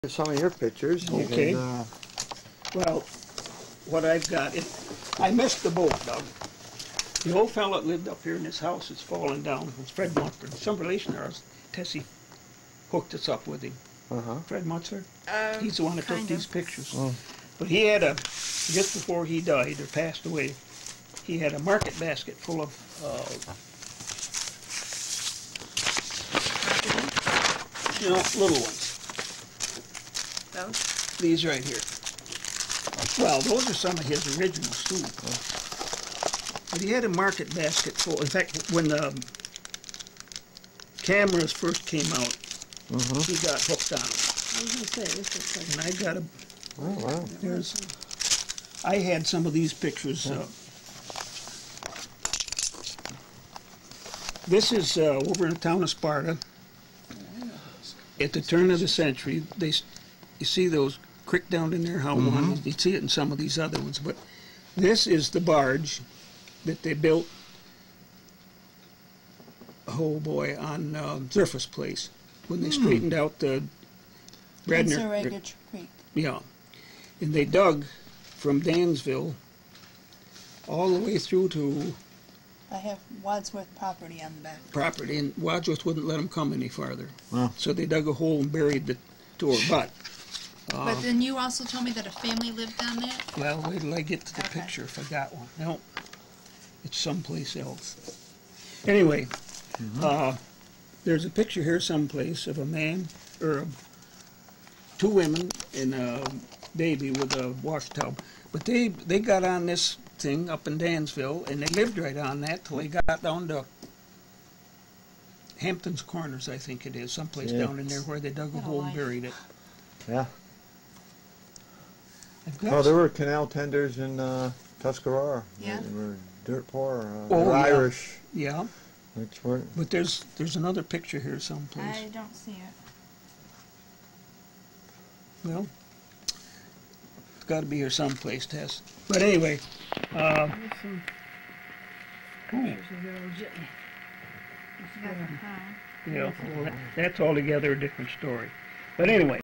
Groveland NY Barge in West Sparta Straighten out Bradner Creek YouTube
Bradner Creek going through the Groveland Flats in West Sparta NY is straightened out using a barge outfitted with a steam shovel.  Bradner Creek sometimes also referred to as Mud Creek has overflowed its banks for many years causing extensive flooding to the farmer's fields and other property.  In this video, Mush Hartmann discussed the work done circa 1943.